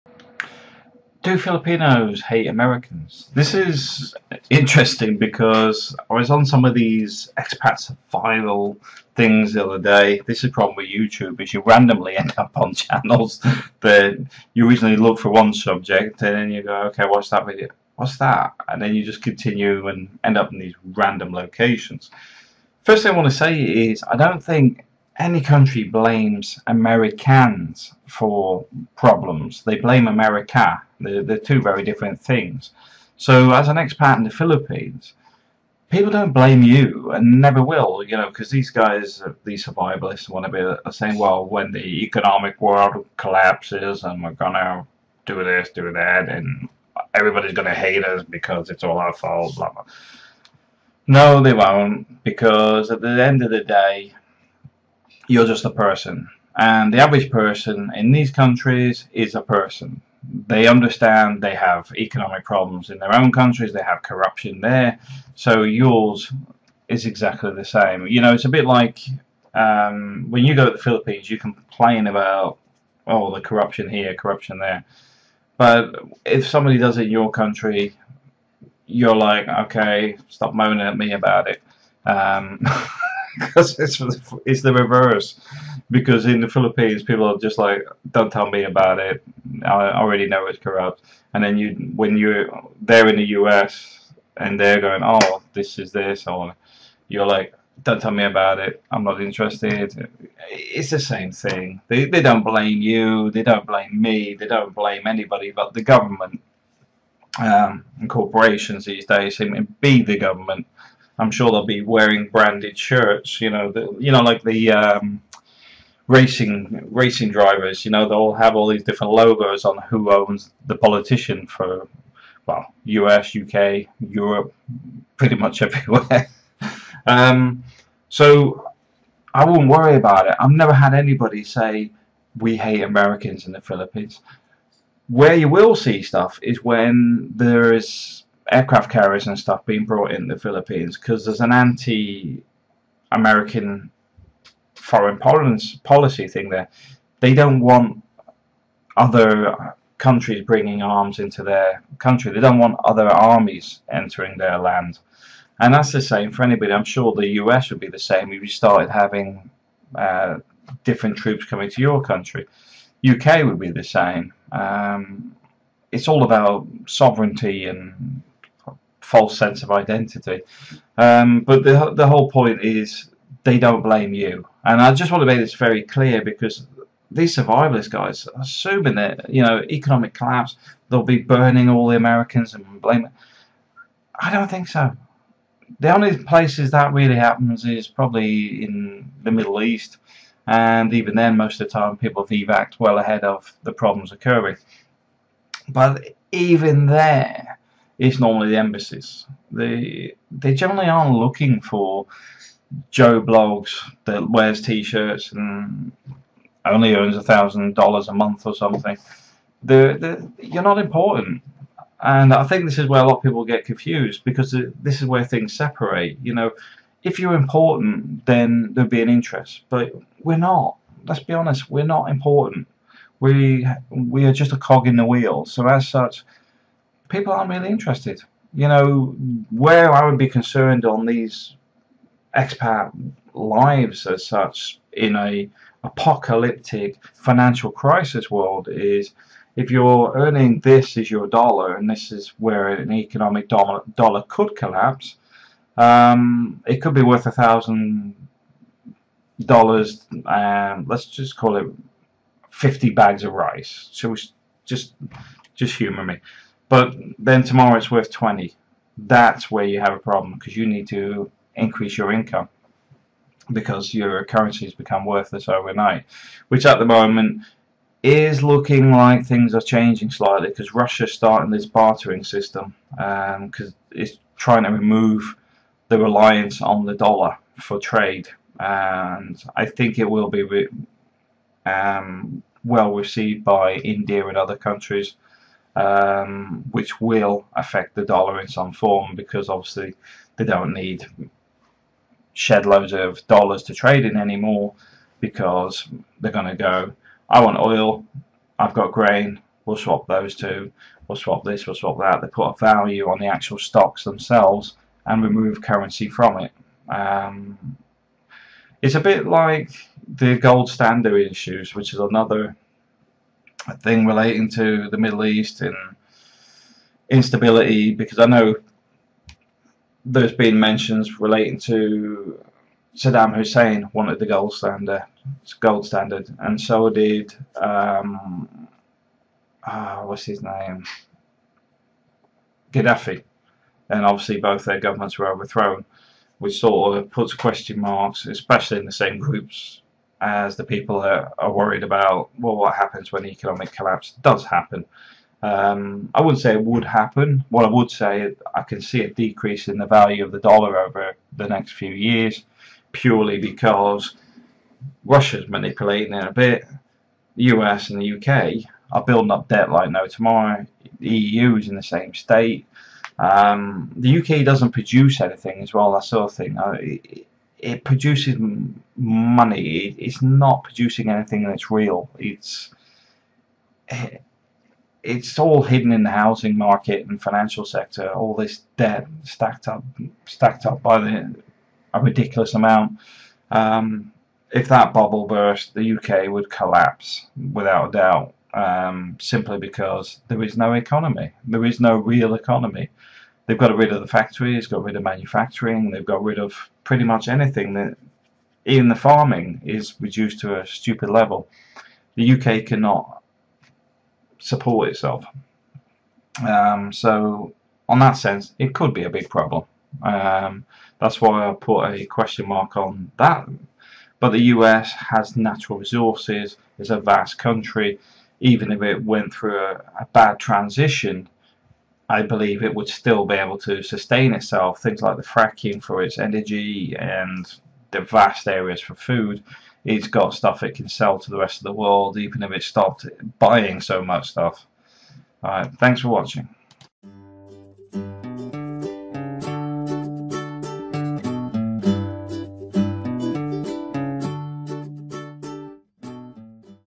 Do filipinos hate americans?
I came across this subject rather randomly on YouTube while looking at something else. Seems the survivalist community and others assume people will hate Americans if the Dollar trade economy collapsed. If people hated Americans they would do already, but like most things people are aware people are simply people! What countries get upto with goverments and corporations etc. is out of the control of the average American or anyone else from another country. As such locals don't blame random people for the problems caused by the U.S. foreign policy but do blame America as a "country".

I wanted to make this very clear as it does seem some people are very paranoid. But in the Philippines all foreigners are simply foreigners although people call everyone "Kano". They often can't tell the difference between an American and someone from anywhere in Europe. Even the newspapers will report "Foreigner" when relating to things Japanese and Korean people have been upto.